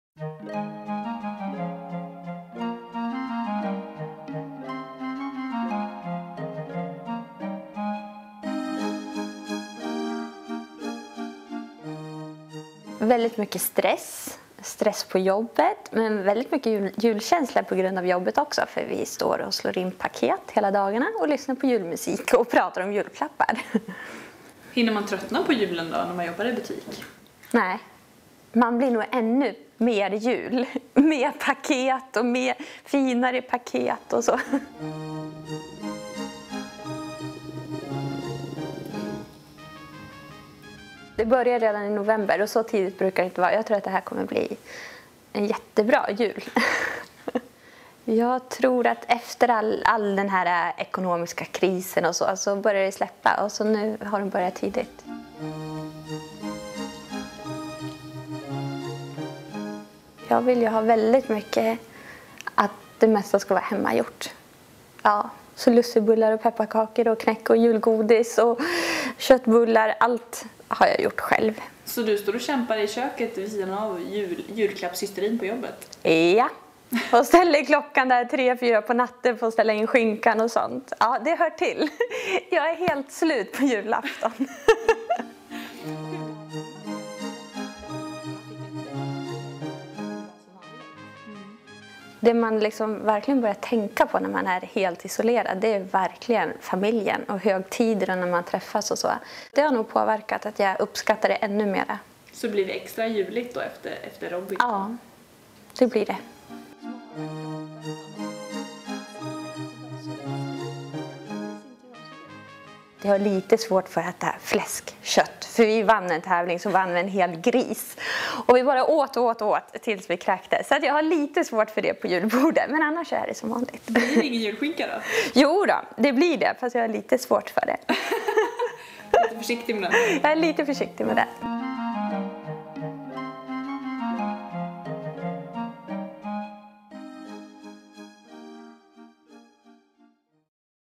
Så här firar en Robinsonvinnare jul
Sedan Ellenor Pierre tog hem Robinson-segern och utsatte sig för diverse konstiga utmaningar och svält har hon börjat vädera hemmet, högtider, och nära och kära på ett helt annat sätt än tidigare.